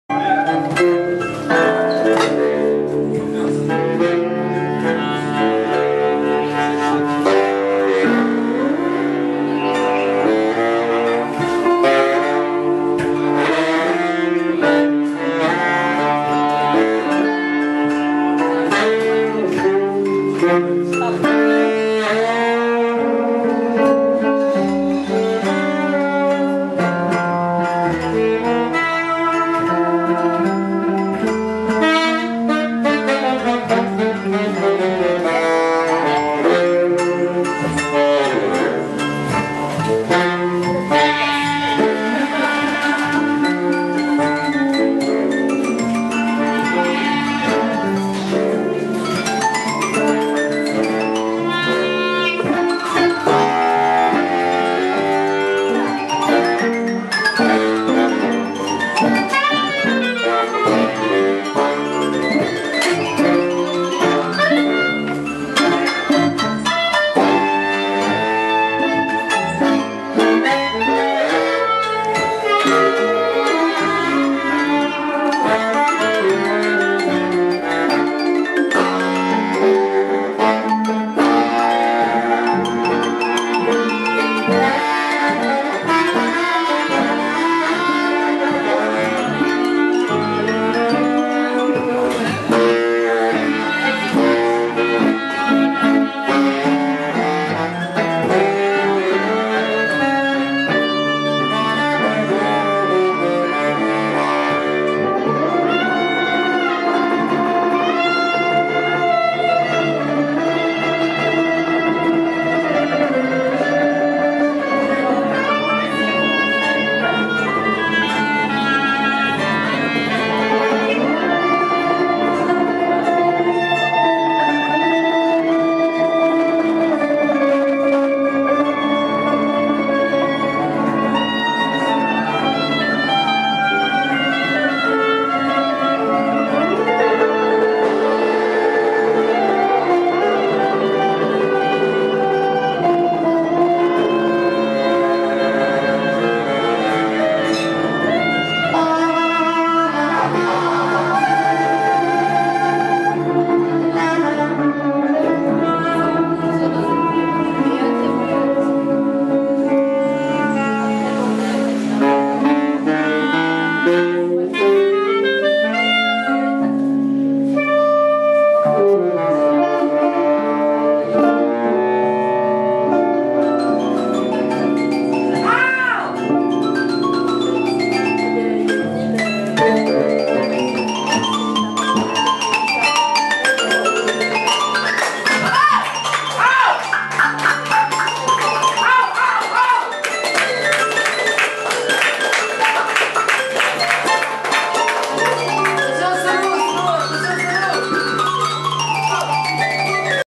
Asile 404 - Concert Noisy Jazz du 13 août 2013
Wojtek Bajda, Piotr Damasiewicz, Jérémie Charras, Erwann Hervé, Pierre-Yves Lawrence